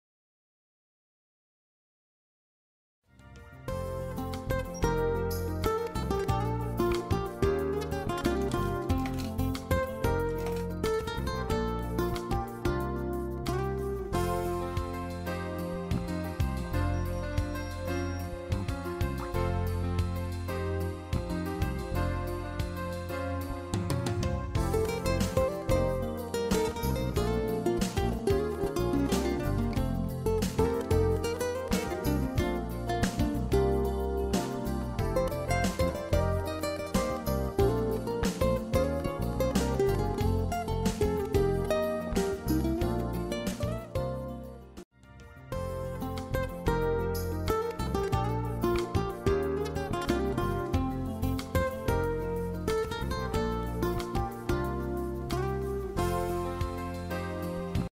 Demo of Fast Charging on Huawei Mate20X via Coin-Operated Cellphone Charger
Cellphone Model: Huawei Mate20X
Battery Capacity: 5000mAH
Charging Station: 4 ports (model: Charger-A206)
Payment Method: Coins
Charging Duration: 10 minutes
Battery Percentage:  6% to 18%

- Skype: chochogz221